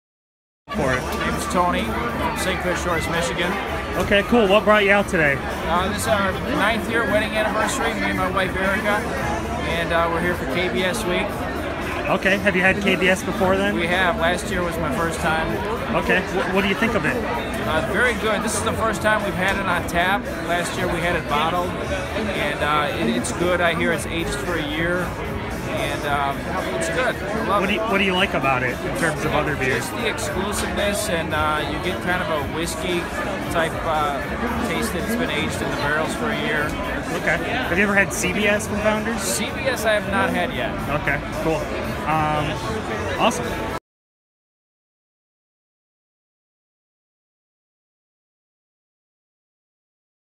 Tony Morabito of St. Clair Shores discusses Founders’ KBS
KBS was released in Founders taproom in Grand Rapids on Saturday, March 10.